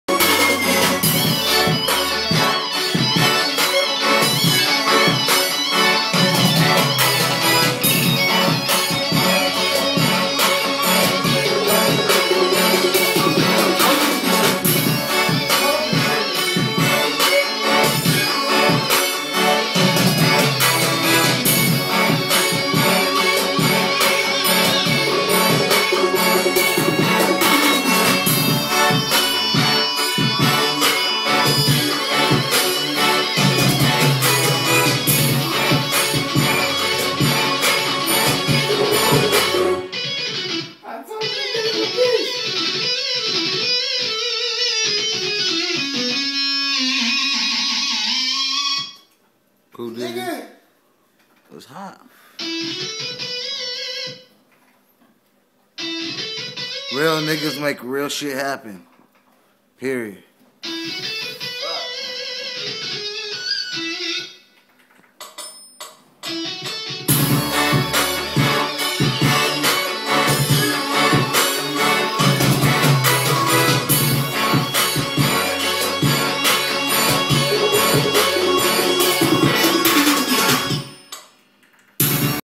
Strict 9 working with Church Boi in studio in Cali
Church boi makes a beat live for Strict 9